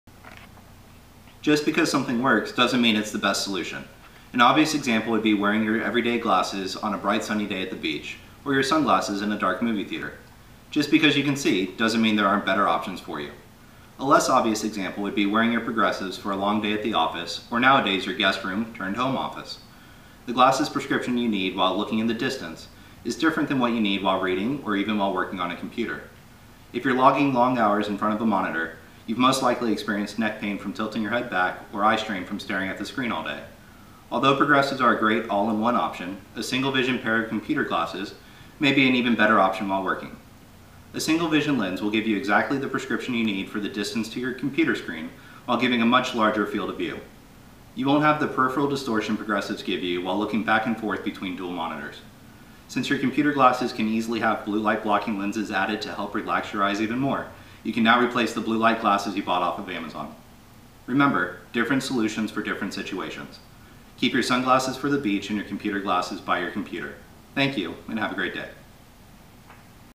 Computer glasses with Doctor Gebhard
Doctor Gebhard talks about the importance of computer glasses and the impact it can have on your daily life!